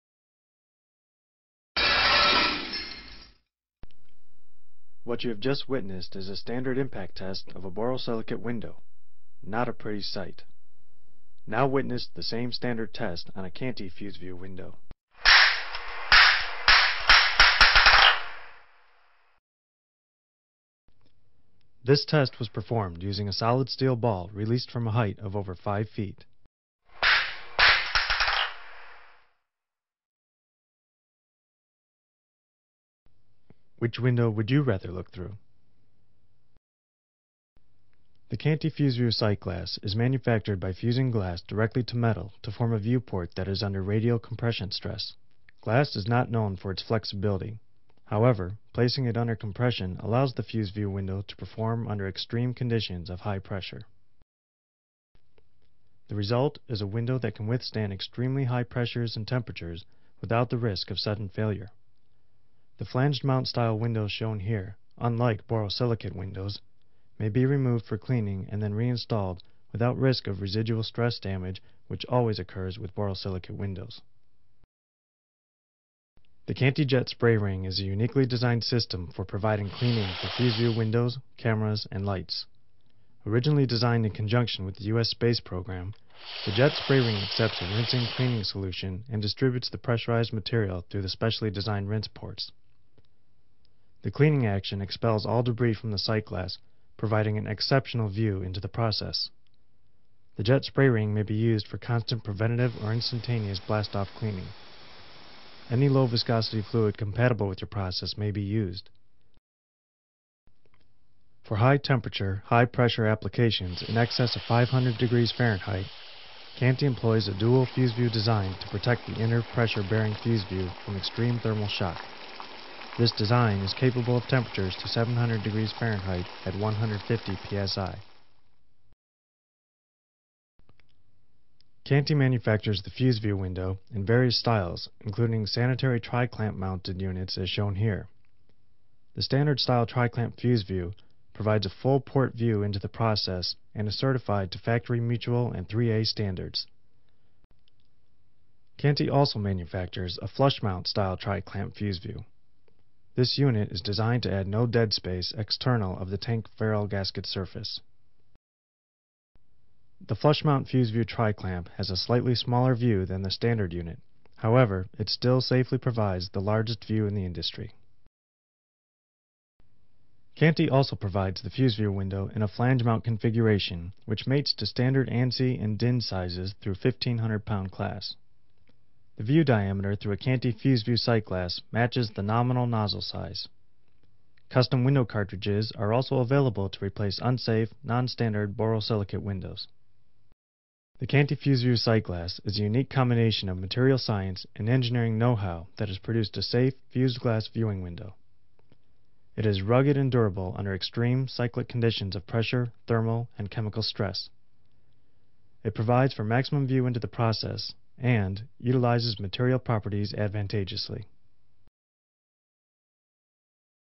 J.M. Canty Fuseview Sight Glass
J.M. Canty Fuseview™ sight glasses have been engineered to meet all your process and safety needs. All standard Fuseviews™ feature Factory Mutual approval and were designed and tested to ensure the safest product available. Canty can provide certification of material and testing if required, typical of ASME code requirements for process vessels.